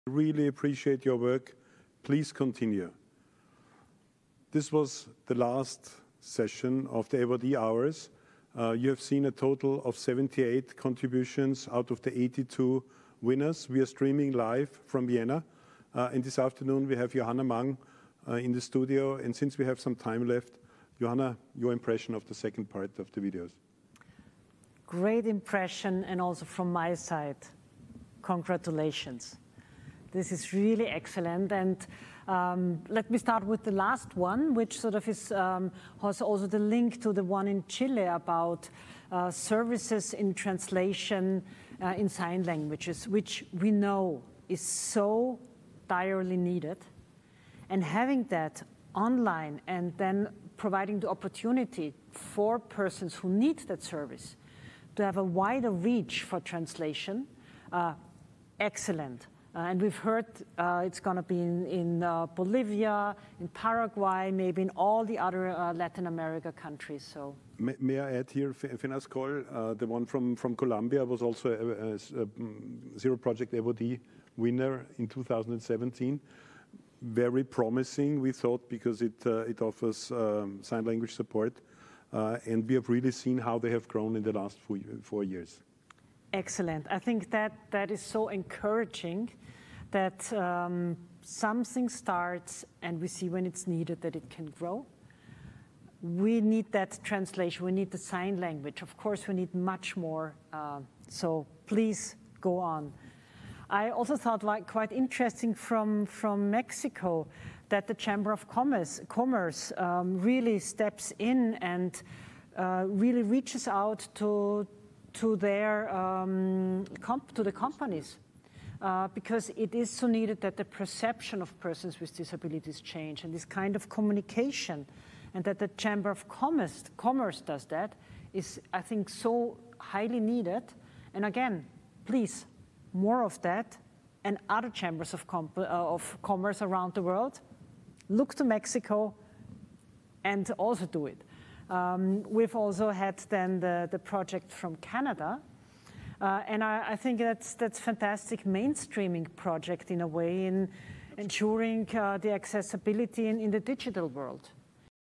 Cierre Zero Project
"Felicitaciones, es realmente un excelente servicio de interpretación a Lengua de Señas y es una oportunidad para las personas que lo necesitan", fueron las palabras de los presentadores de Zero Project, en relación al reconocimiento recibido entregado al Centro de Relevo Colombia